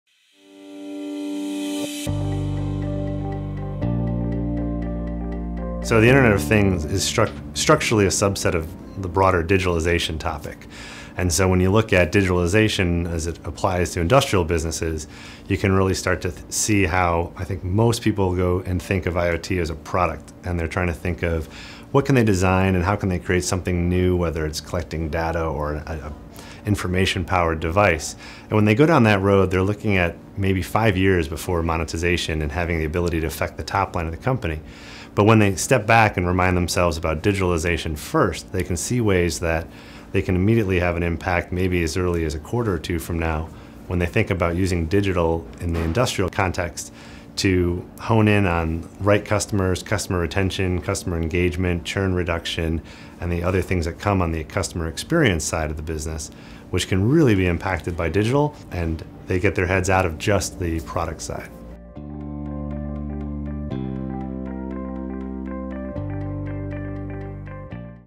Monetizing Digitalization in Industrials
The internet of things (IoT) is becoming an attractive business opportunity for industrial companies. However, they've mainly been focusing on IoT as a product. Digitalization expert Adam Echter shares an alternative approach.